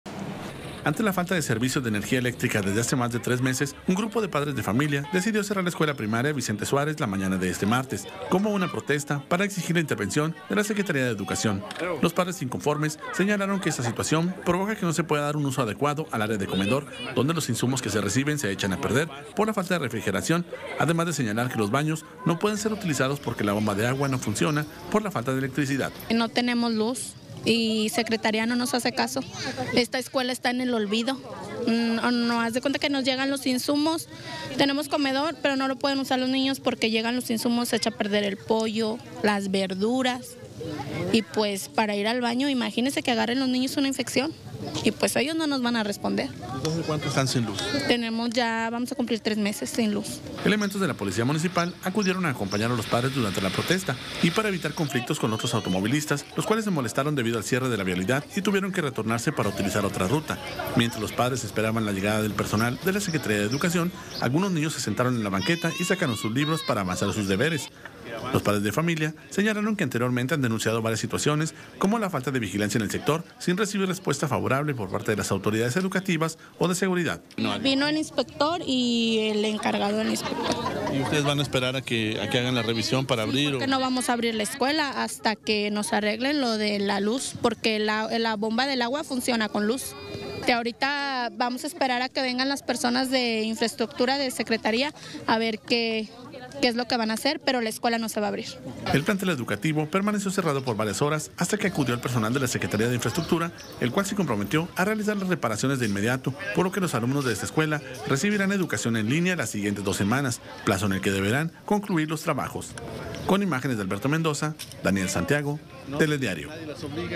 Cierran escuela ante falta de electricidad en Saltillo
Ante la falta de servicio de energía eléctrica desde hace más de tres meses, un grupo de padres de familia decidió cerrar la escuela primaria Vicente Suárez, la mañana de este martes, como una protesta para exigir la intervención de la Secretaría de Educación.

Los padres inconformes, señalaron que esta situación provoca que no se pueda dar un uso adecuado al área de comedor, donde los insumos que se reciben se echan a perder por la falta de refrigeración, además de que los baños no pueden ser utilizados porque la bomba de agua no funciona por la falta de electricidad.

Programación
Lunes a viernes:
Telediario Matutino de 06:00- 10:00
Telediario Mediodía de 12:00-14:00